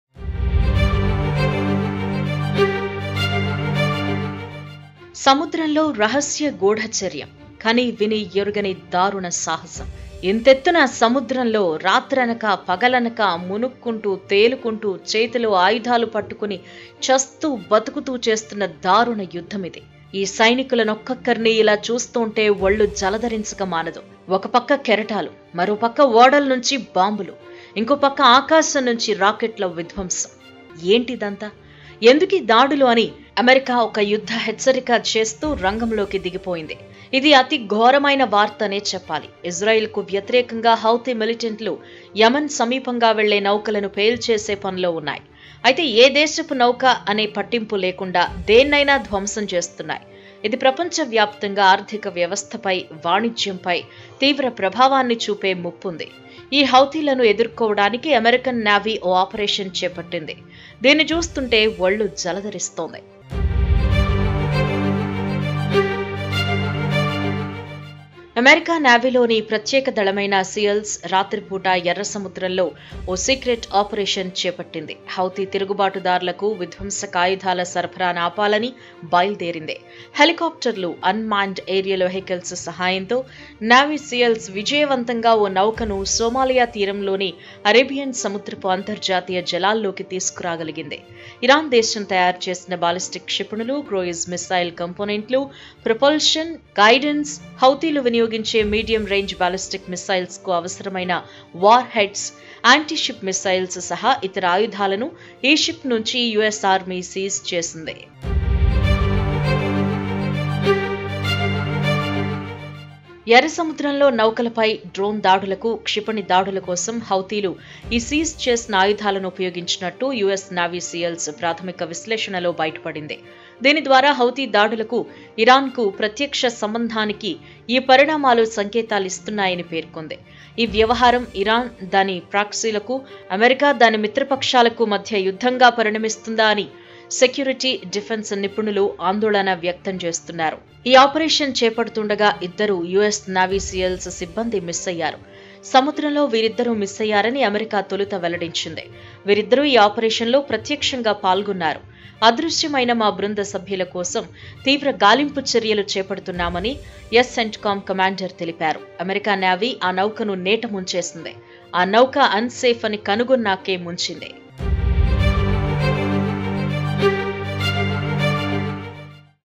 సముద్రంలో సీక్రెట్ కోవర్ట్ ఆపరేషన్. అంతా ఘోరం || US secret operation || ABN Digital
సముద్రంలో సీక్రెట్ కోవర్ట్ ఆపరేషన్. అంతా ఘోరం || Pentagon Announces 'Operation Prosperity Guardian' to Stop Attacks by Houthis || ABN Digital